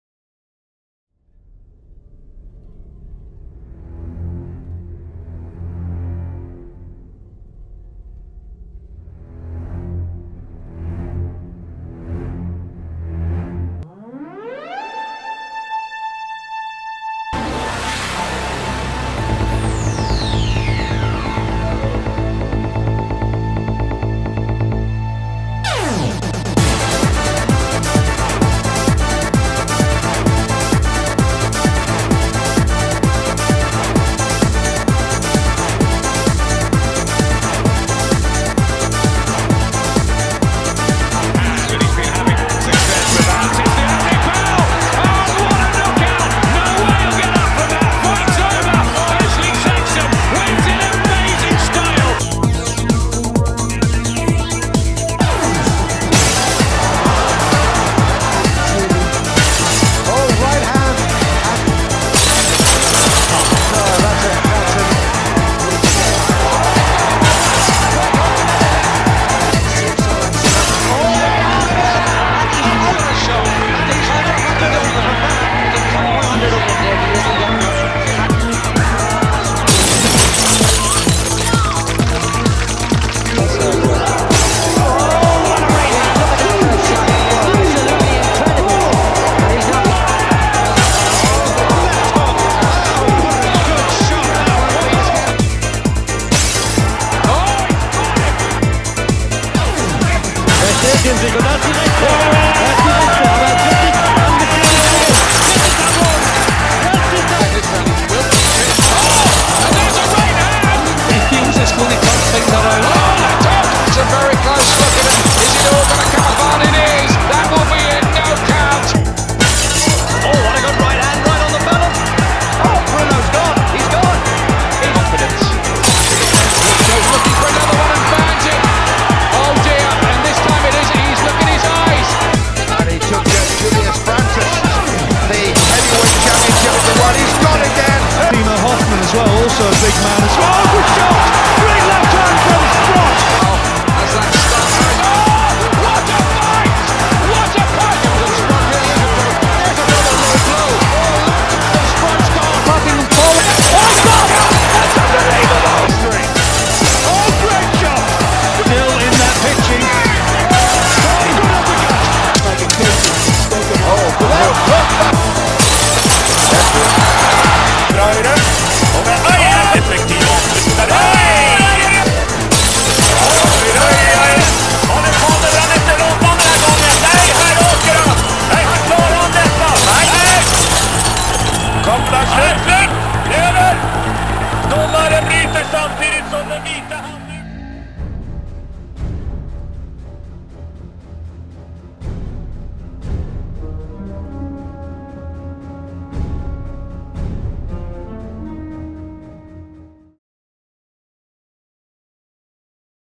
Glass Jaws: The Movie - The Brits
British boxers getting their glass jaws shattered.  Watch the china shards go all over the ring!!!